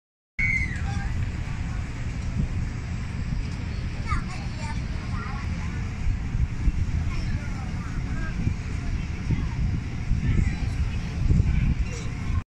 Singapore - view of Malacca Strait
This short clip was filmed from the top of the Marina Bay Sands in Singapore.